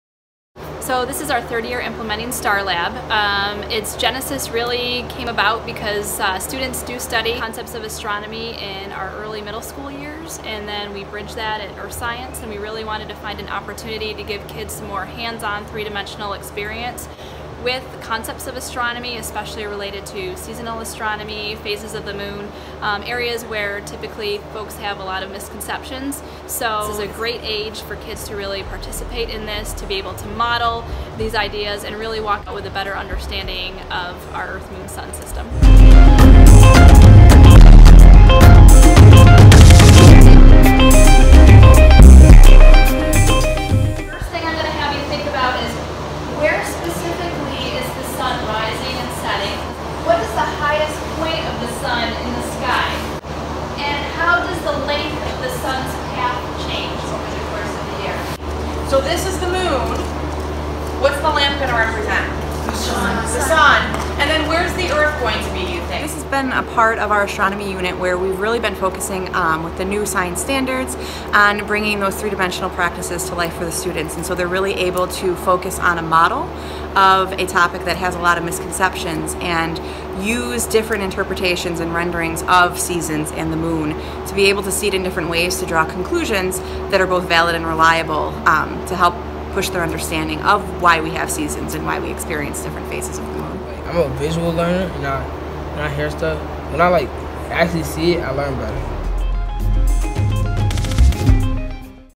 Visual learning via ‘StarLab’
Dake Junior High School students got to see phases of the moon and learn more about the sun’s path through the sky in “SkyLab.”